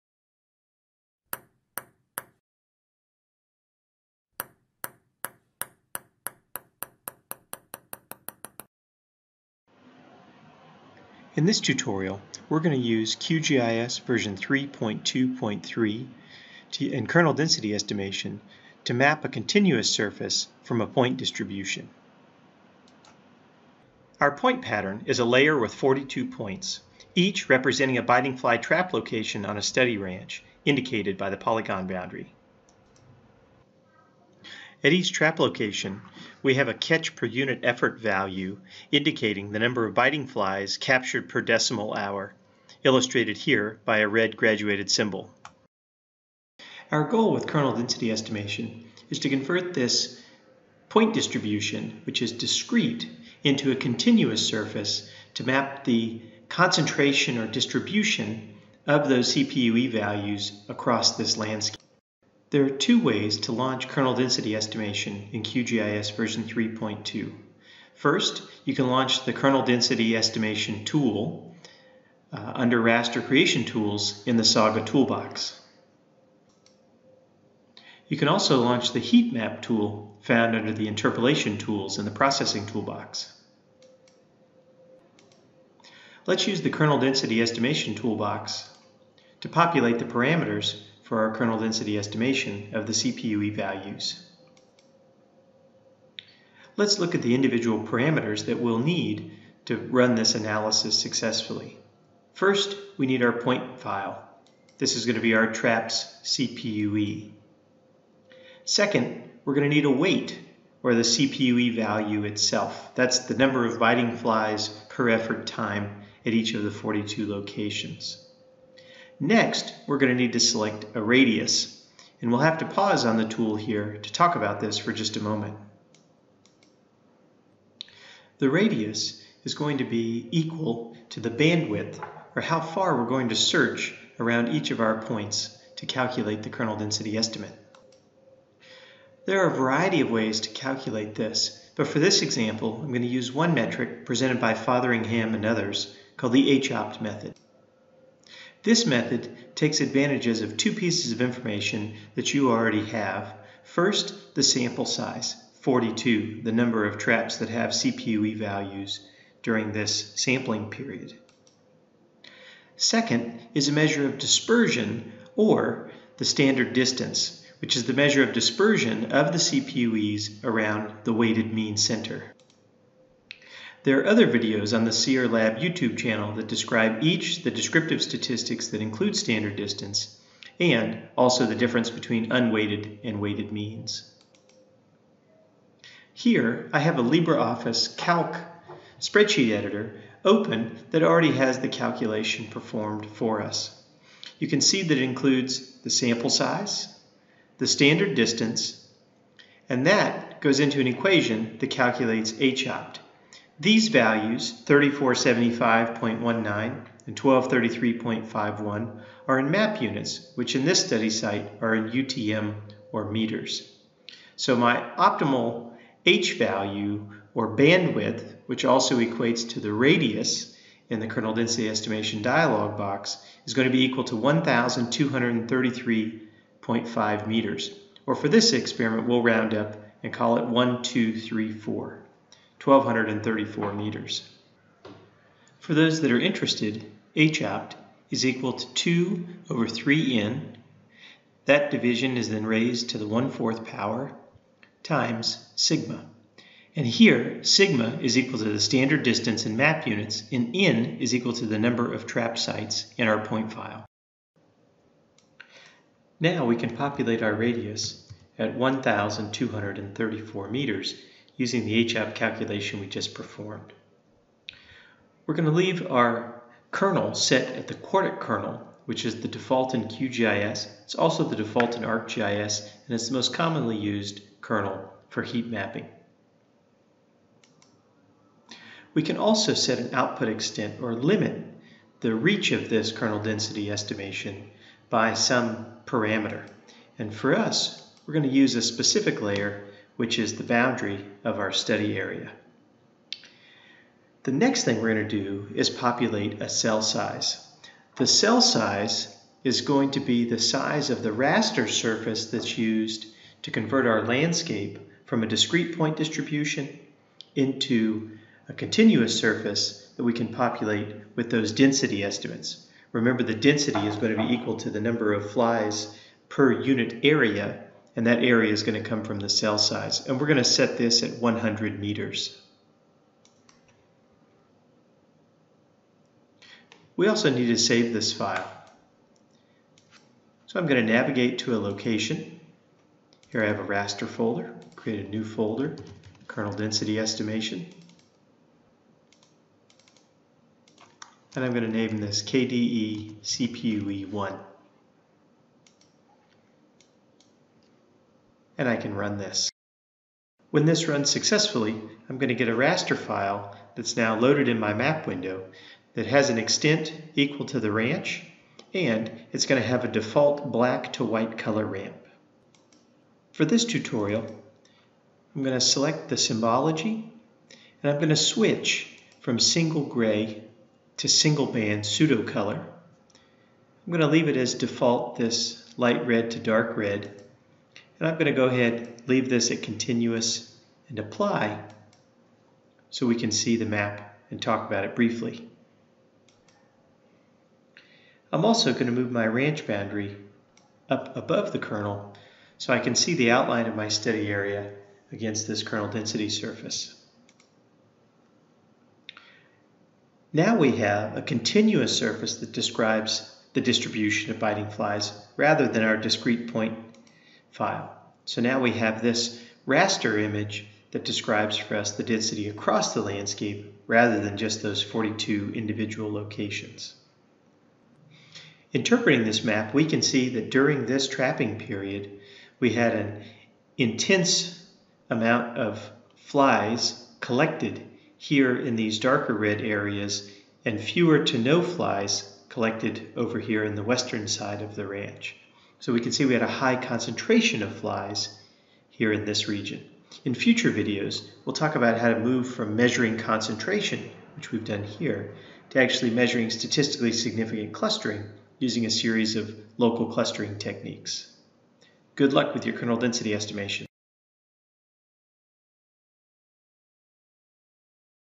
Kernel Density Estimation with Q-GIS 3.2.3
This tutorial illustrates how to perform kernel density estimation in Q-GIS 3.2.3 and calculate the bandwidth using the optimal h value described by Fotheringham and others.

The full reference for hopt can be found here:

Fotheringham, A.S., C. Brunsdon, and M. Charlton. Quantitative Geography: Perspectives on Spatial Data Analysis. Sage Publications Ltd, 2000.

The data in this video were orginally published by Blackburn and others (2004) in the Annals of the Association of American Geographers.